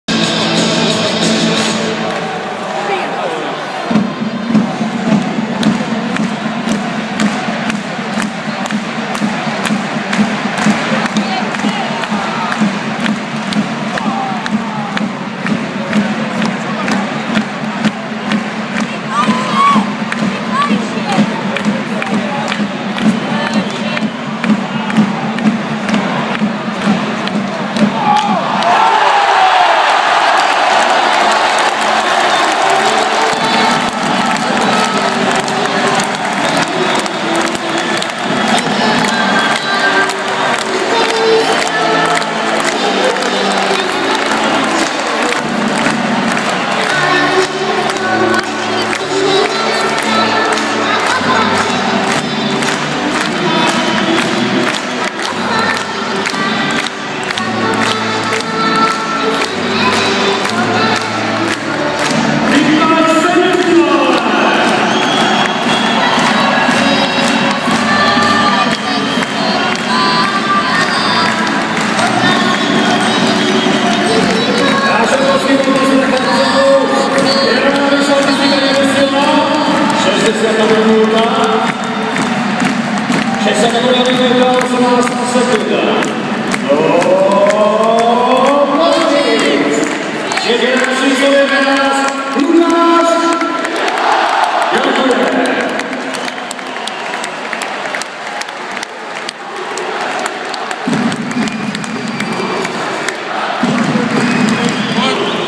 HC Košice - HC 05 Banská Bystrica 2:1 po predĺžení (0:0, 1:1, 0:0 - 1:0) rozhodujúci gól
rozhodujúci gól